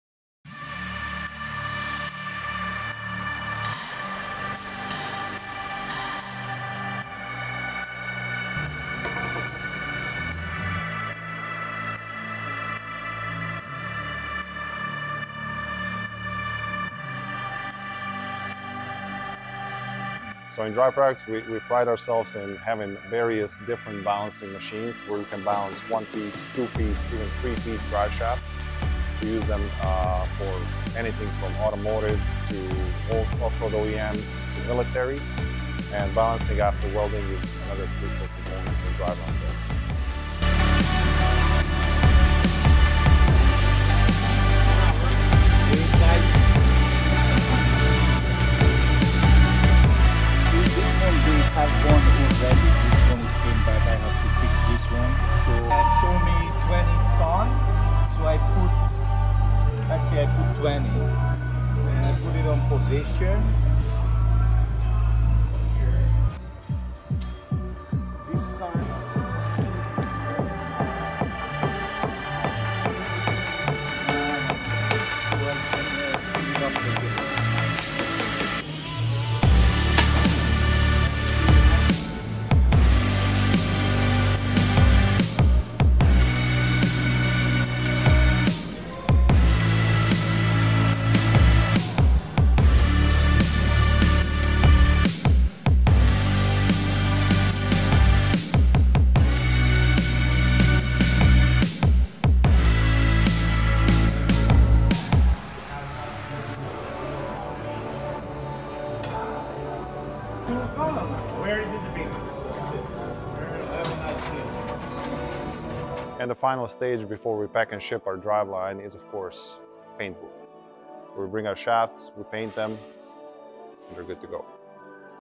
Balancing a Driveshaft with Drive Products Driveline
Whether it's a new build or a driveshaft in for repair and maintenance, balancing the driveshaft is the final step in this process, ensuring your equipment works efficiently. Take a look as our Toronto Driveline team balances a driveshaft in for maintenance.